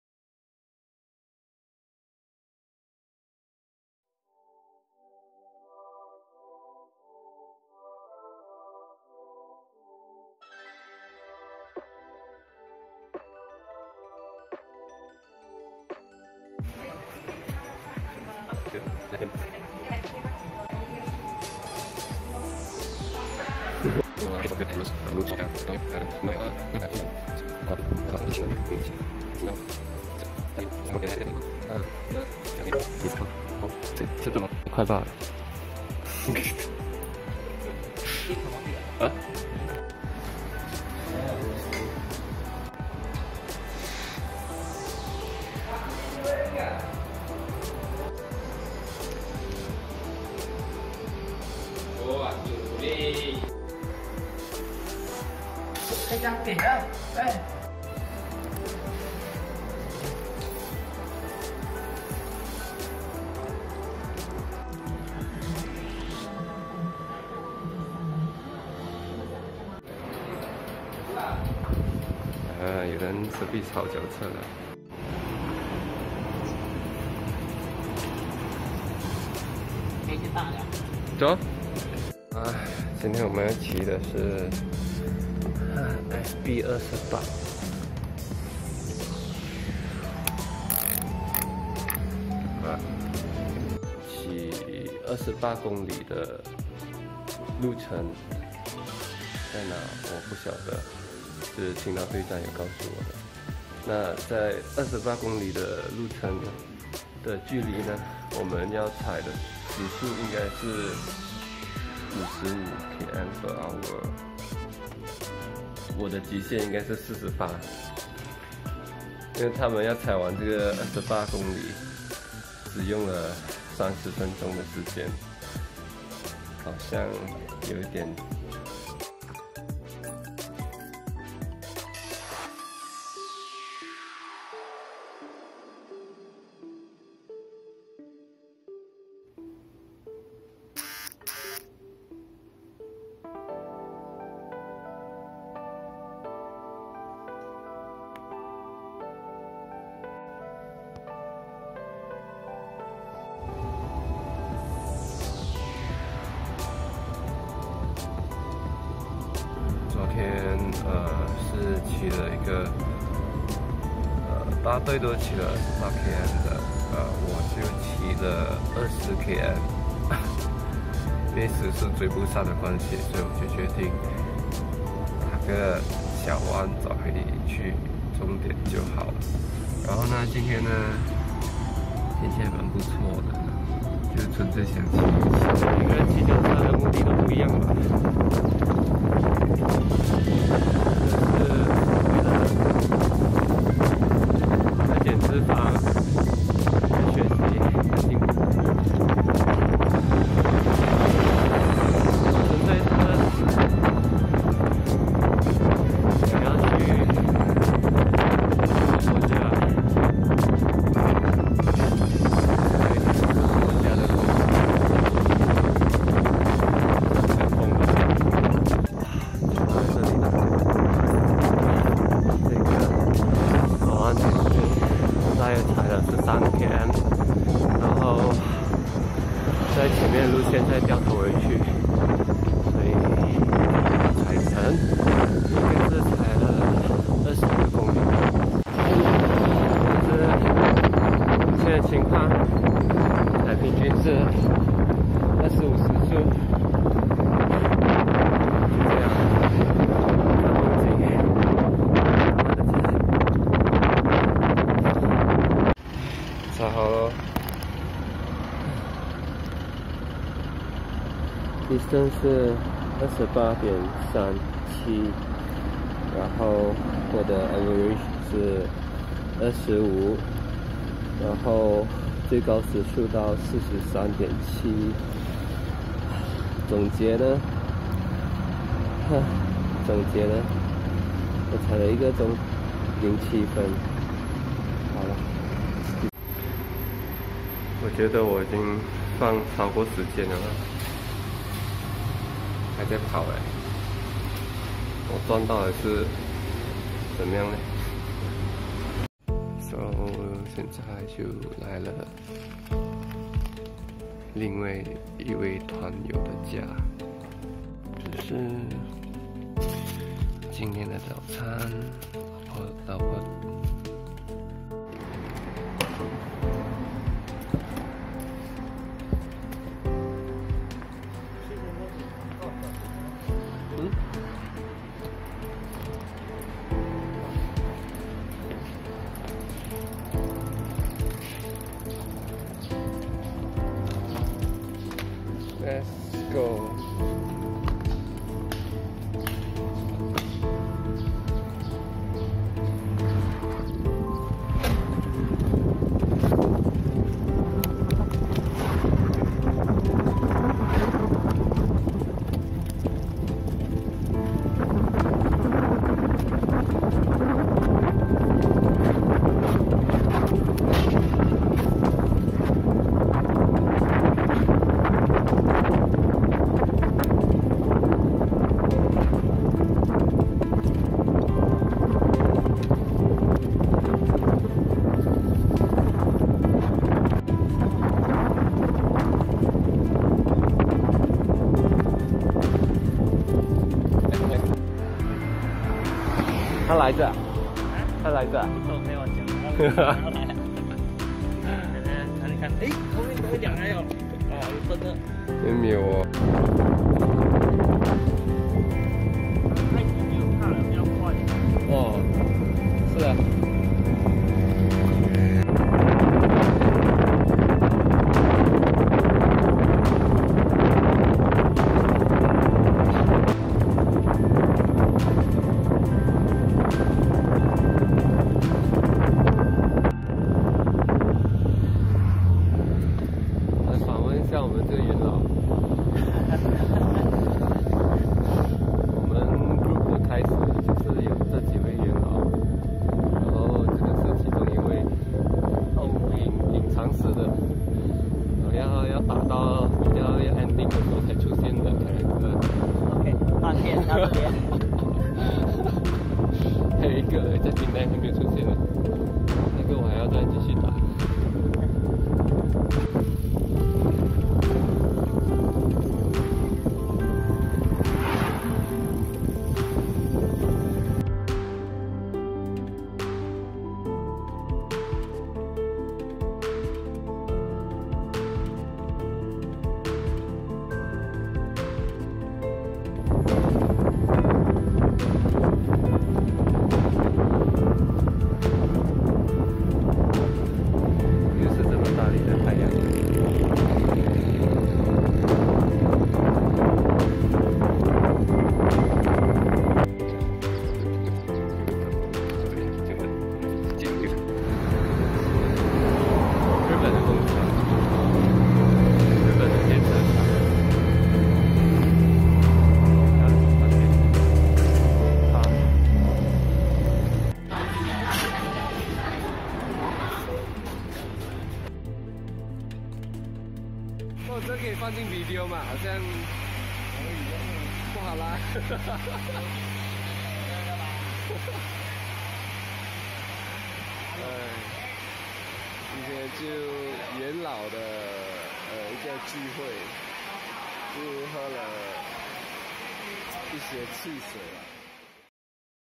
原创单车[Cycling VLOG] - 吉兰丹 | SP28 + 高路 + 市区 76km | 平淡六月第一周
原本说好的jeram pasu泡汤了，
内疚的队长请喝汽水来补偿他的缺席 XD。
欢迎Tu Ming的归来，
依然照旧的Ah Huat来陪骑，谢谢！